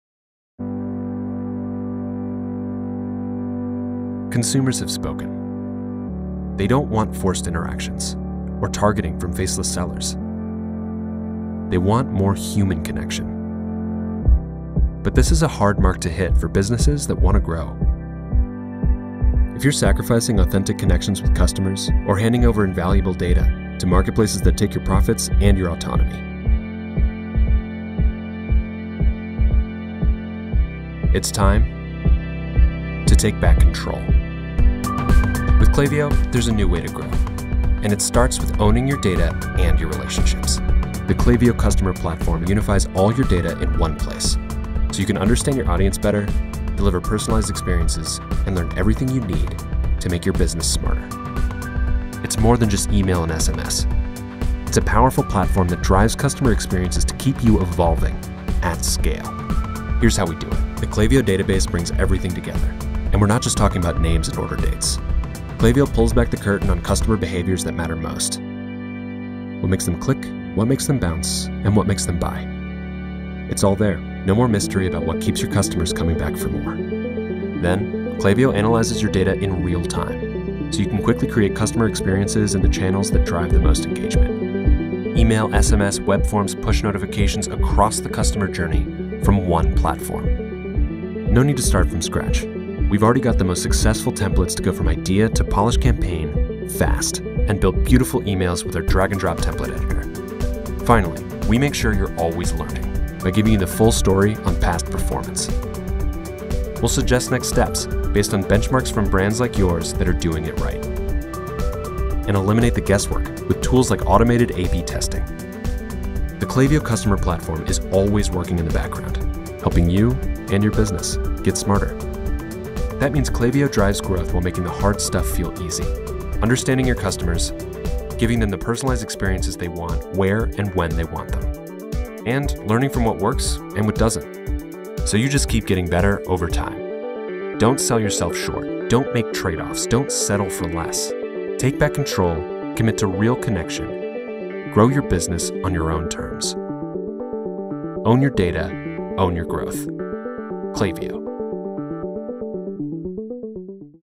Email Marketing & SMS Made Easy with Klaviyo
Klaviyo: Own Your Customer Data & Grow Smarter.
Struggling to break through the noise and truly connect with your customers? Tired of fragmented marketing tools and unreliable data?

There's a better way.

Klaviyo, the leading Customer Data Platform (CDP) for eCommerce, empowers you to own your data and craft personalized customer experiences across email marketing, SMS marketing, and more.

Unify Your Customer Data:

Ditch the silos! Klaviyo centralizes all your customer information, including purchase history, website behavior, and preferences.
Understand your audience like never before. See what makes them tick and tailor your marketing accordingly.
Effortless Personalization:

Drag-and-drop email and SMS builder lets you create targeted campaigns in minutes.
Leverage pre-built templates and Klaviyo's insights to craft high-converting messages.
Marketing Automation Made Easy:

Automate abandoned cart emails, welcome series, and other key touchpoints for consistent customer engagement.
Klaviyo analyzes data in real-time, suggesting the best channels and timing for your campaigns.
Stop Guessing, Start Growing:

A/B testing and performance reports ensure you're always optimizing your campaigns for maximum impact.
Learn from industry benchmarks and Klaviyo's expert recommendations to accelerate your growth.
Take Back Control of Your Marketing:

Klaviyo puts you in the driver's seat. Own your data, build stronger customer relationships, and unlock sustainable business growth.